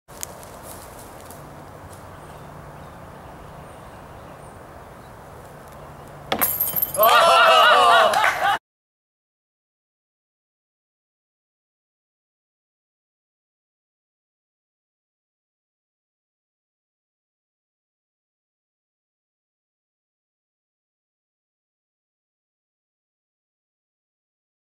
Bank Shot at Texas Army Trail Disc Golf Course
Bank shot off the tree for a par